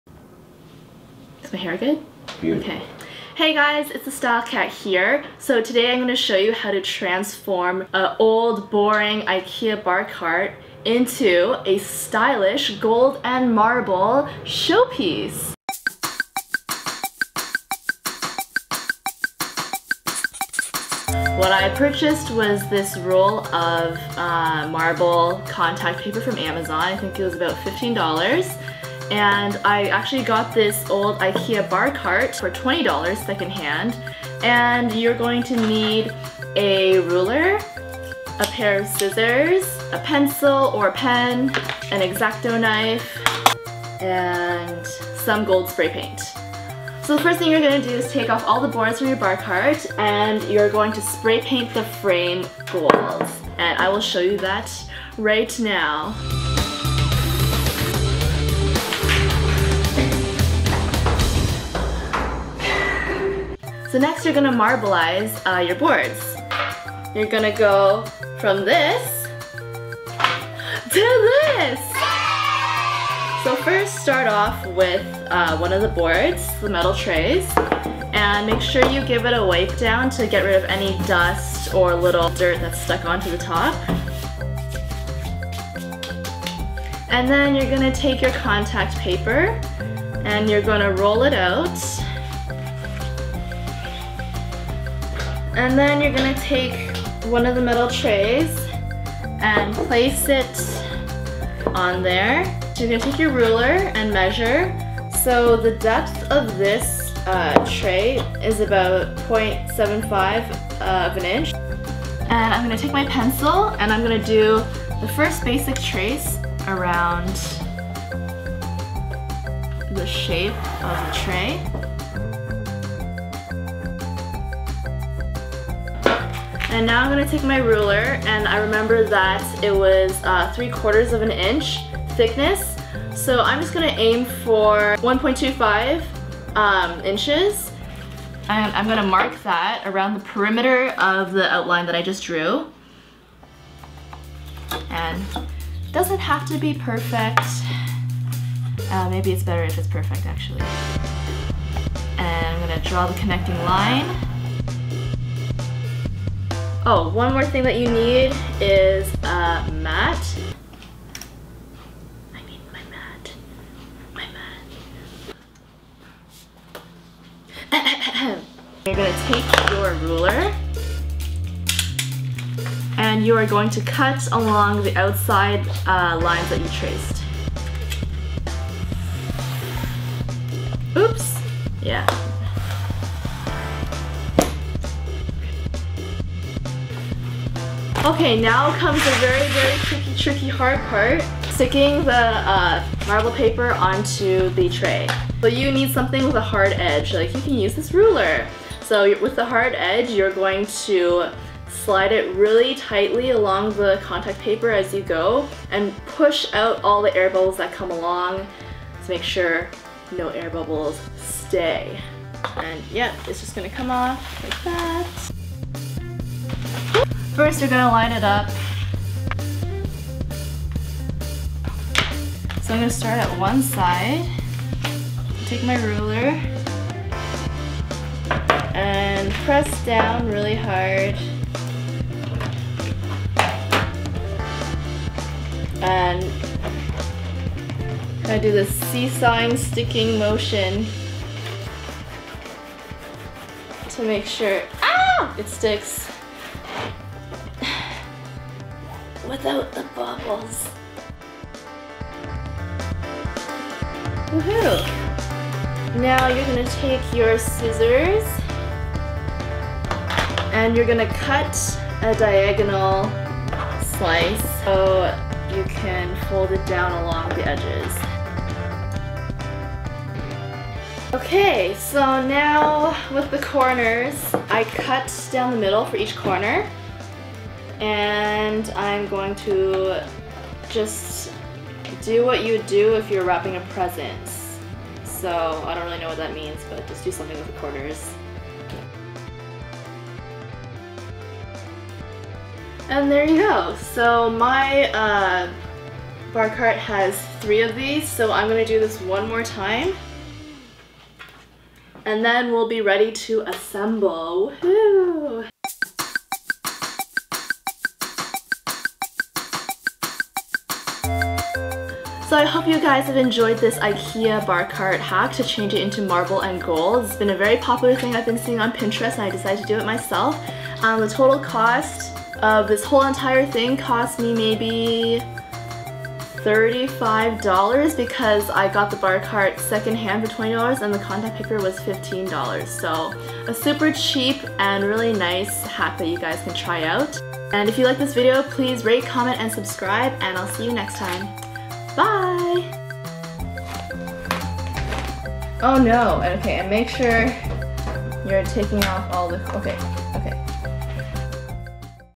IKEA BAR CART HACK: Marble and Gold DIY ($35)
Hi frands! Welcome to The Style Cat's first video of 2017 - and it's a DIY! Up your home decor game with this easy, simple hack to elevate your cheap IKEA Bygel bar cart by adding marble and gold accents. Much style. So wow.

MATERIALS
Marble Contact Paper
Gold Spray Paint
Ikea Bygel Bar Cart